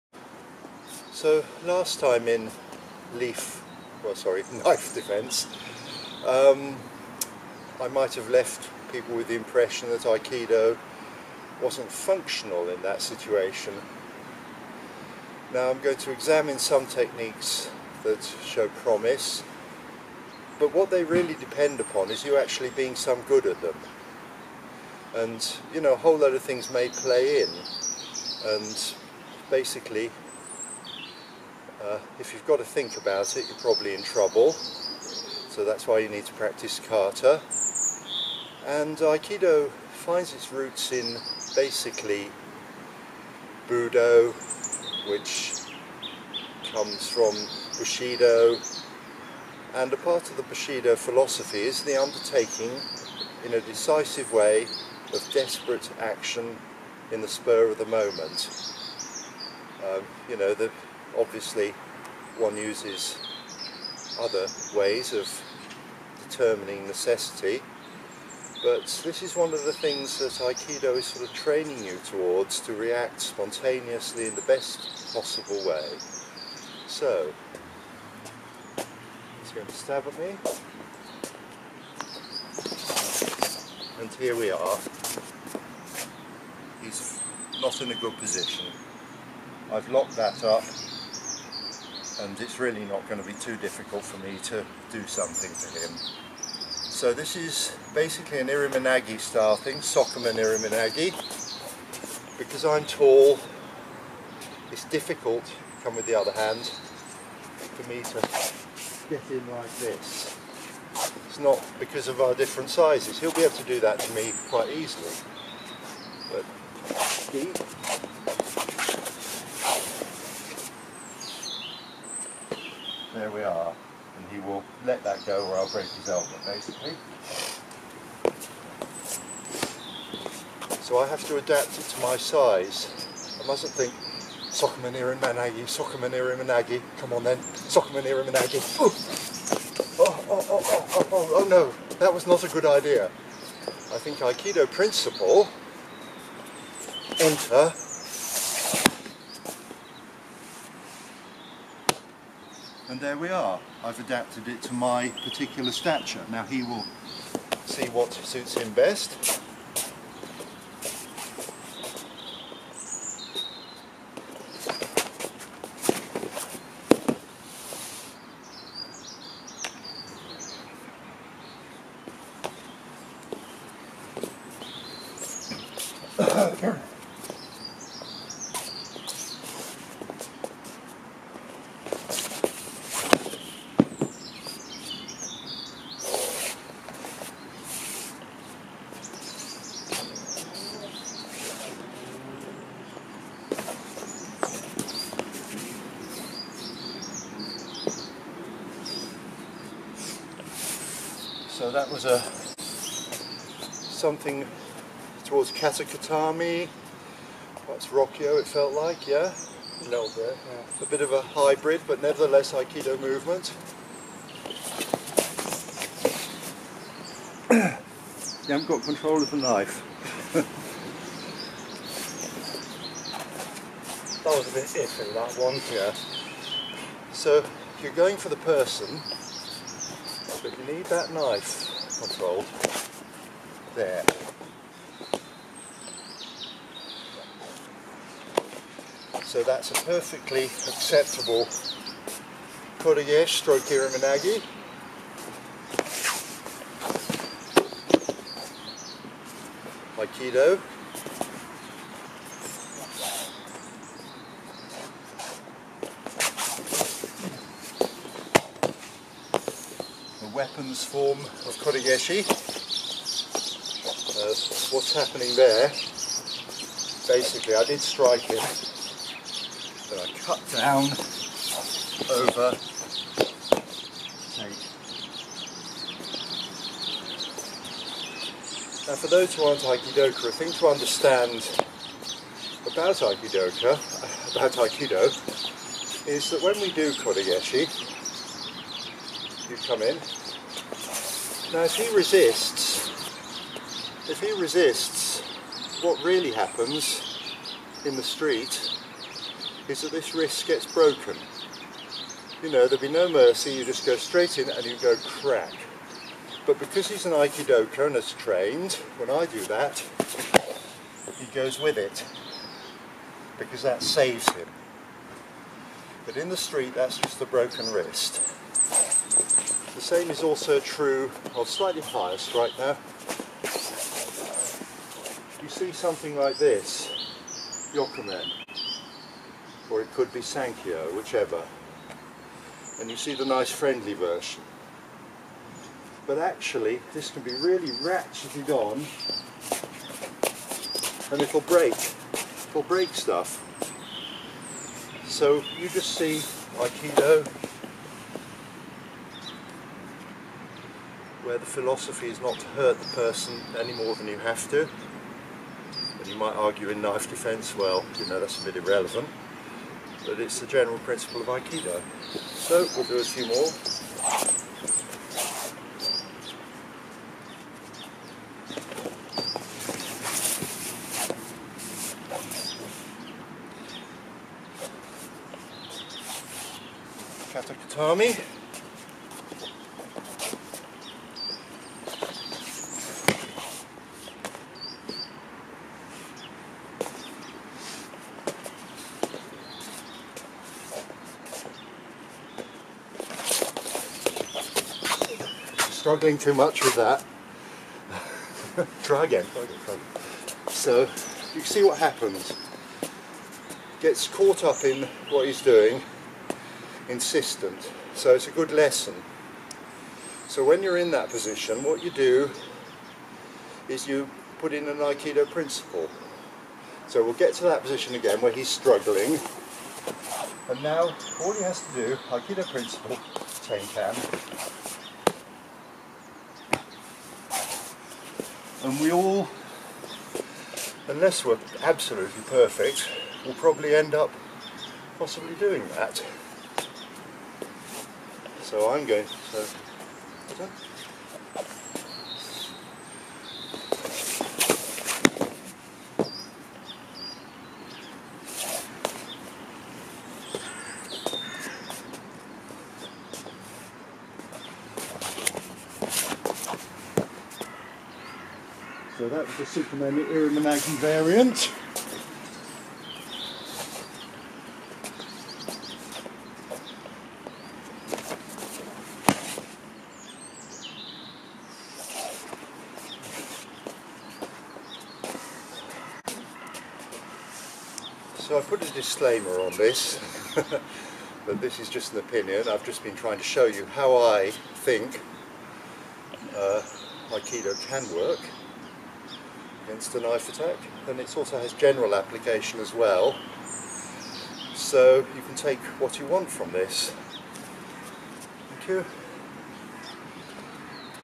Aikido Knife defence #3/ Hereford
More practical Aikido techniques with potential for knife defence, referencing more general Aikido concepts. Shot on the 5th day of Christmas so five gold rings.